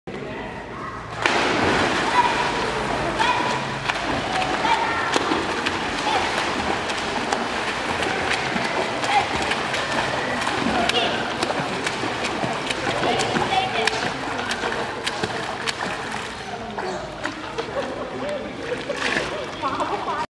Shashu Sharath Swimming Nov 2018
A kid and a ma having a race in which the child wins